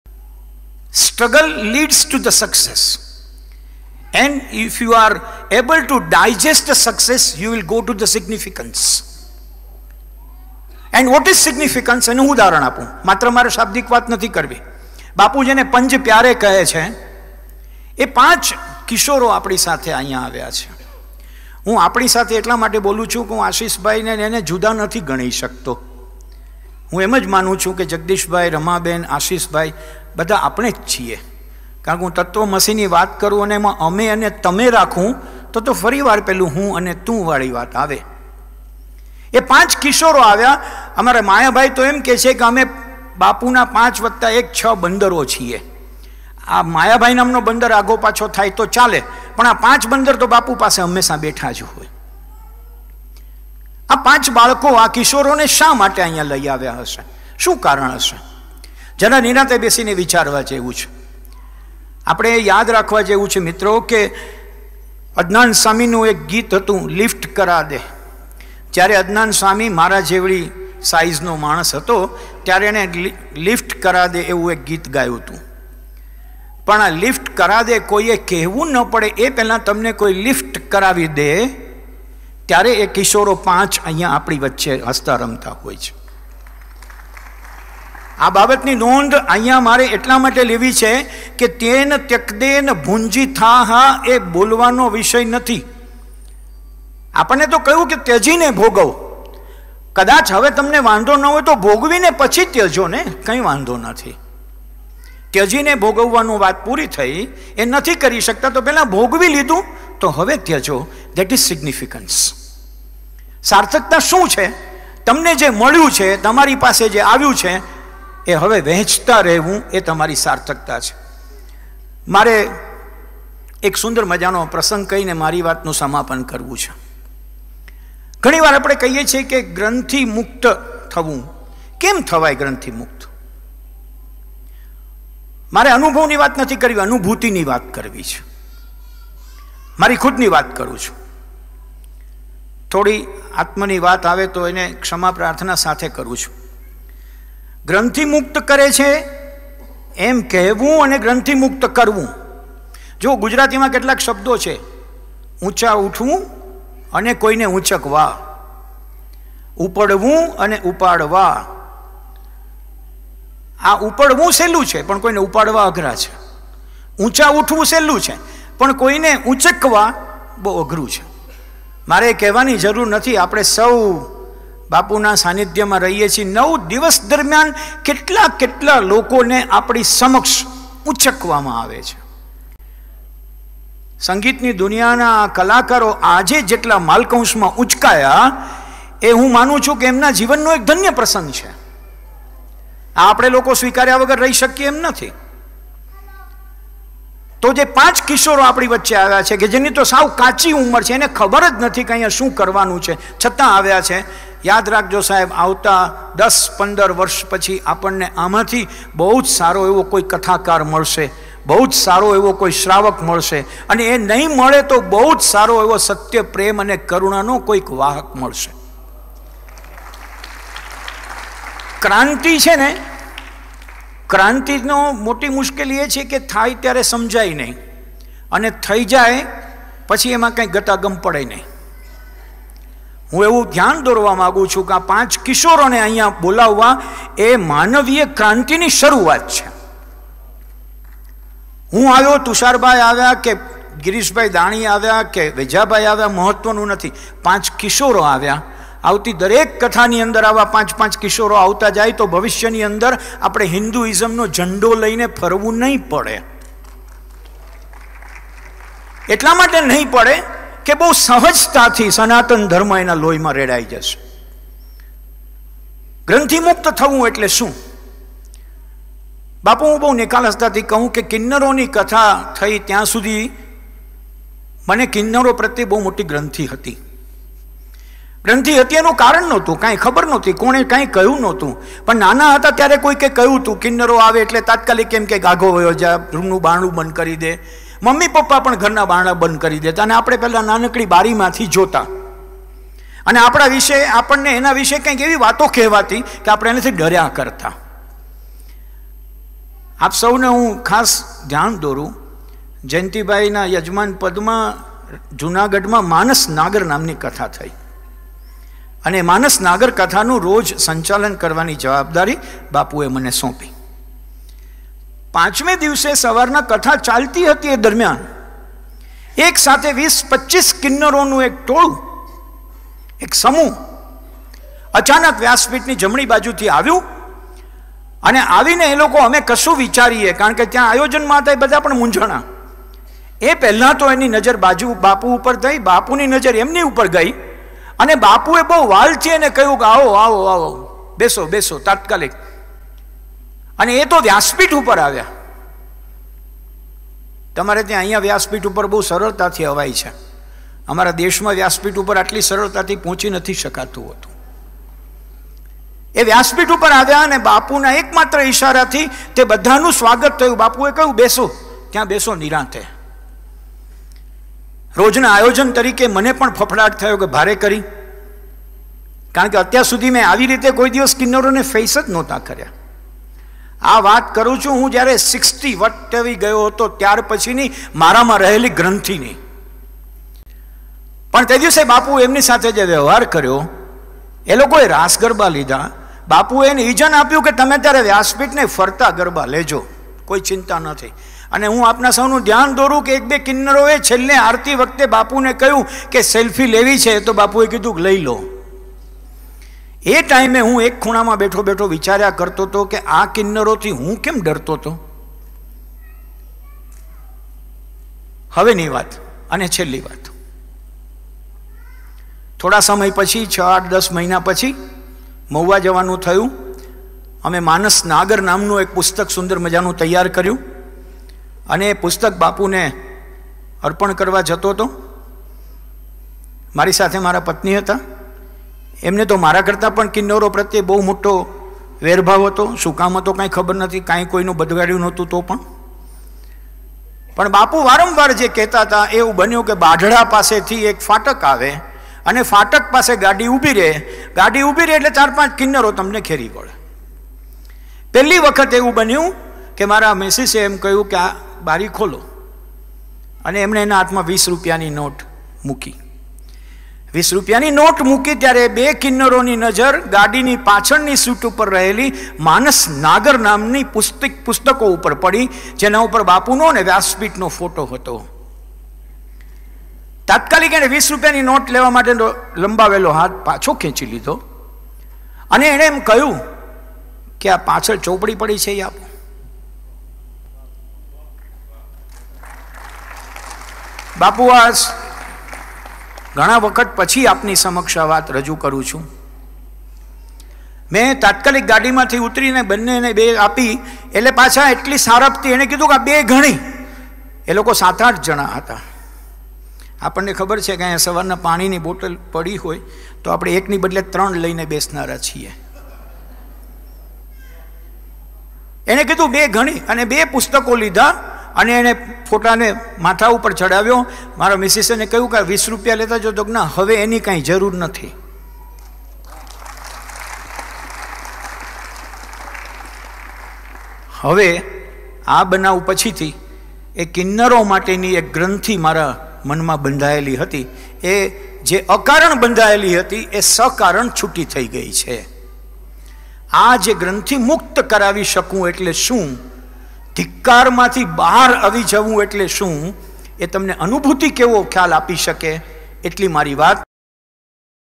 ||Bhadrayu@Science of Living|| khabarchhe:fb પર...(41)
શ્રેણીનો એકતાળીસમો મણકો :: કથામાં કિન્નરો આવી ચડયા, બાપુએ કહ્યું આવો, આવો, આવો..

Bhadrayu Vachhrajani